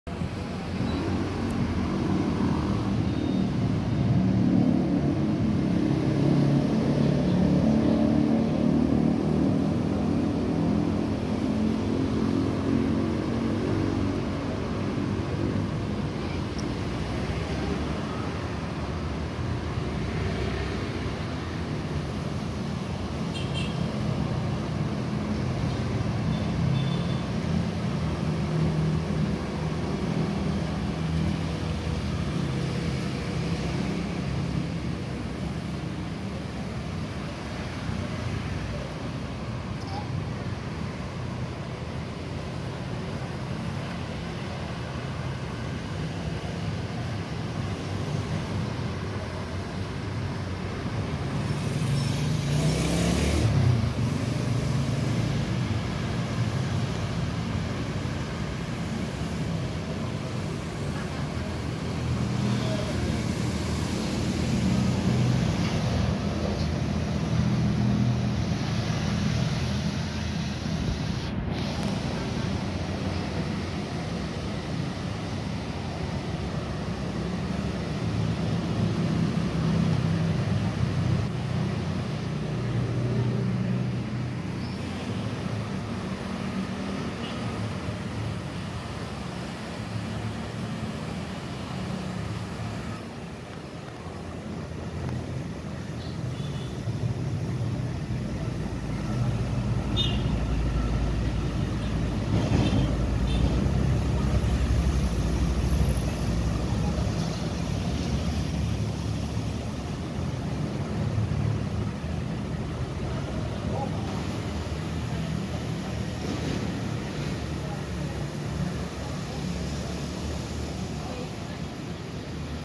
blue moon 30 agustus 2023 @ sahrul munir
fenomena alam bulan kelihatan nampak lebih besar
lokasi vidio disurabaya 19.35 wib
puncaknya jam 20.35 wib.